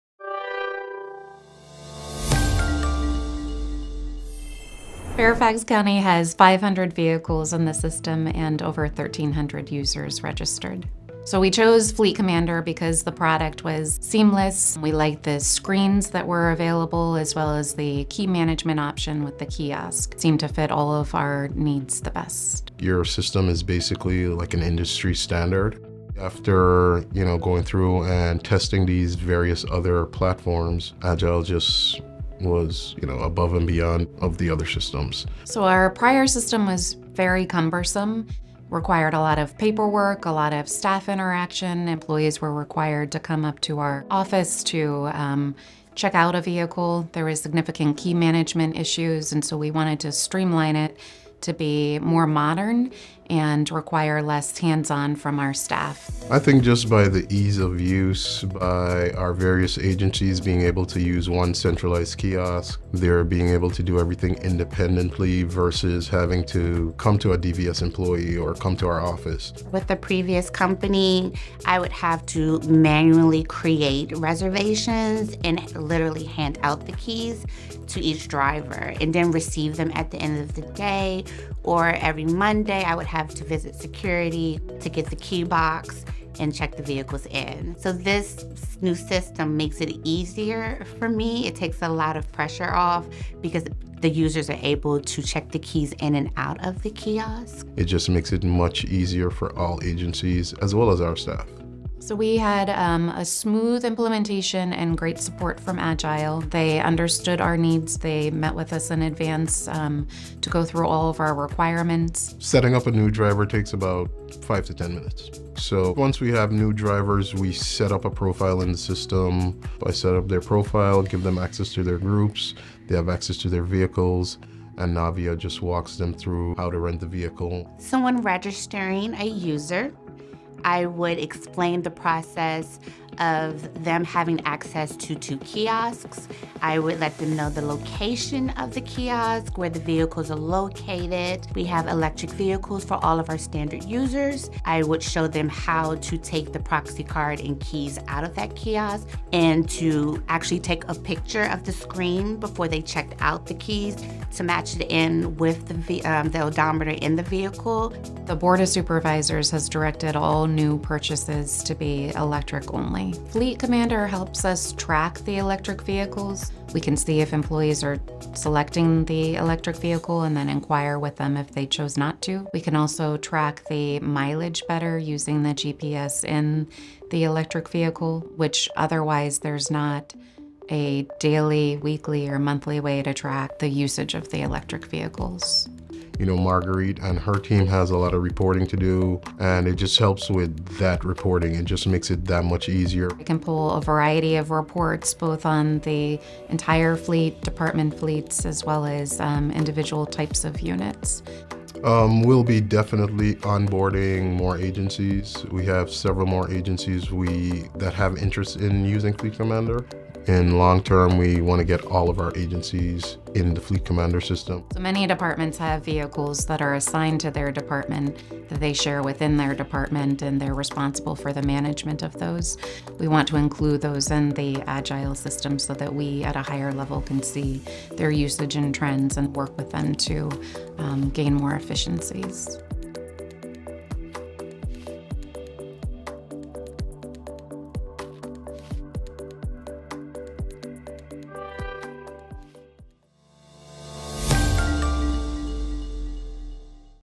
Fairfax County Department of Vehicle Services - Agile Fleet FleetCommander Testimonial
In this video, the Fairfax County Government Department of Vehicle Services describes their experience with Agile Fleet’s FleetCommander.  DVS discusses their experience with automated vehicle sharing and key control, as well as efficiencies gained through reductions in administrative overhead and paperwork.  The team shares how data insights help with driving EV adoption and how reporting helps the team make informed decisions about the composition of the fleet.